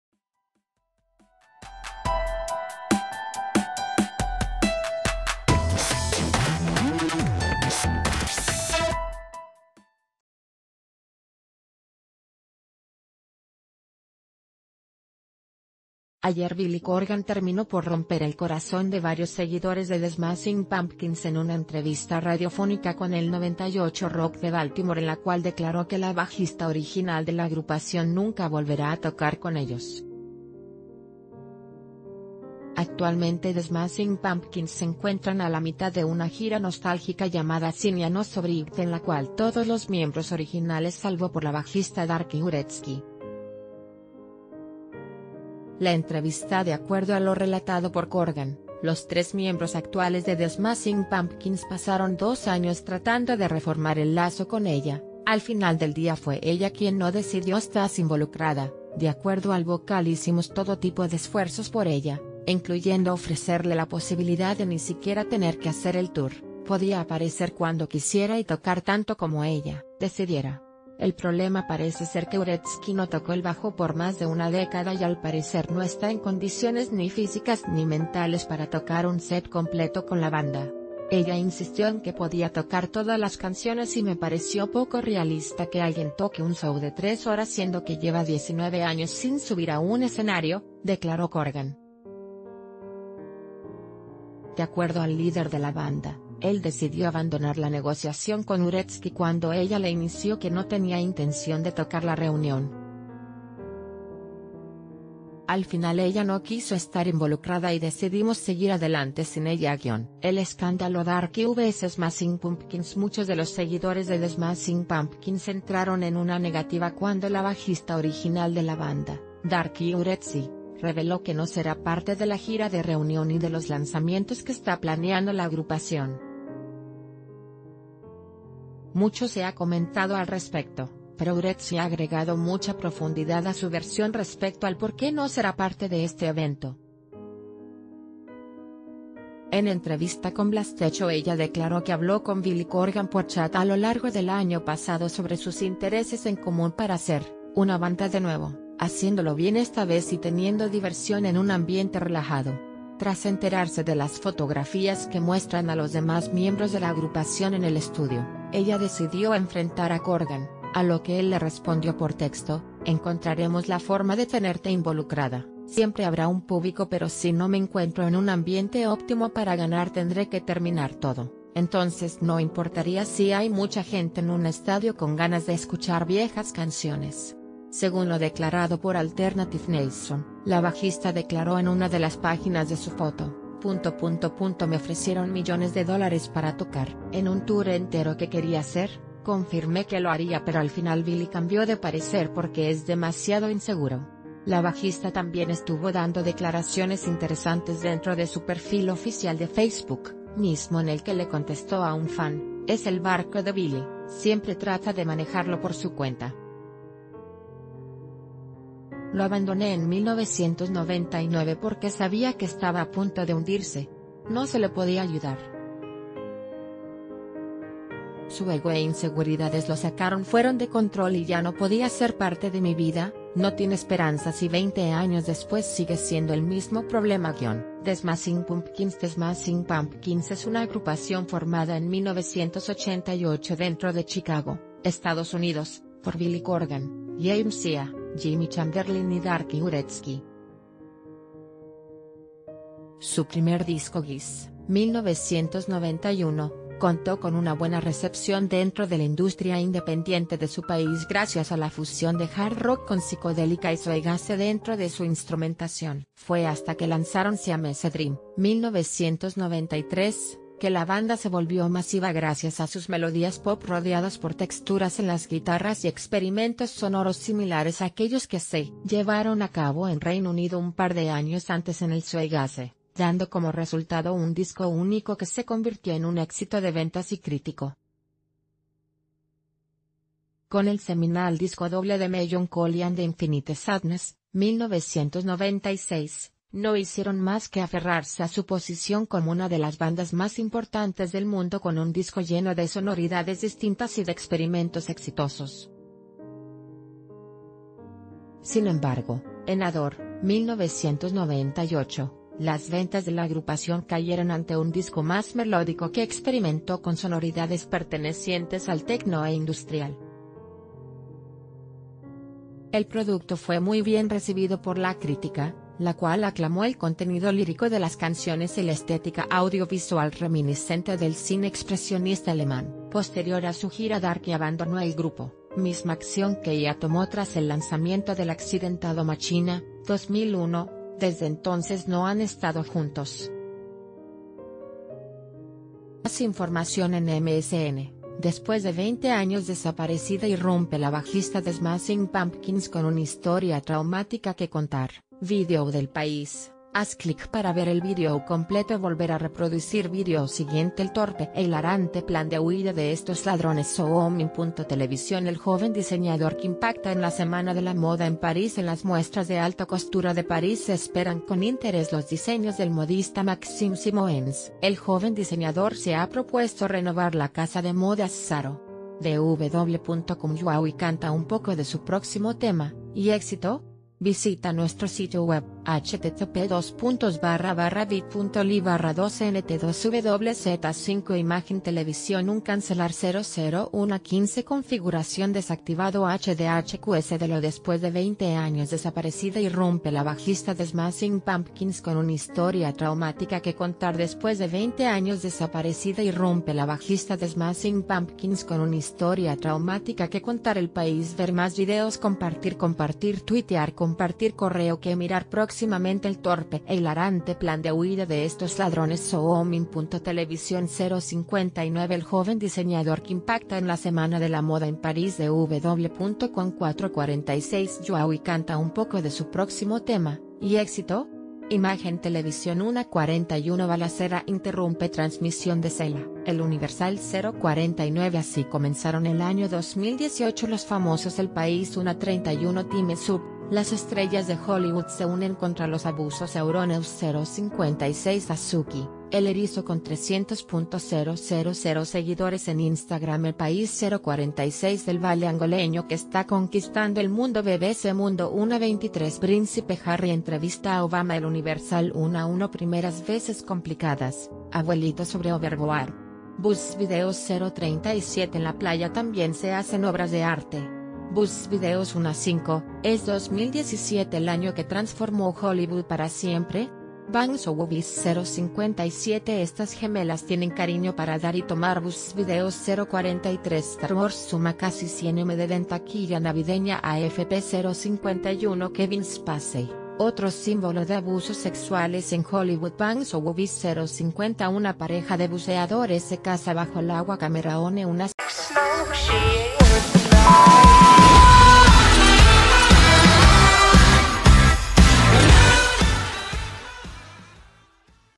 Billy Corgan: “D’arcy Wretzky nunca volverá a ser parte de The Smashing Pumpkins”
Ayer Billy Corgan terminó por romper el corazón de varios seguidores de The Smashing Pumpkins en una entrevista radiofónica con el 98 Rock de Baltimore en la cual declaró que la bajista original de la agrupación nunca volverá a tocar con ellos. Actualmente The Smashing Pumpkins se encuentran a la mi...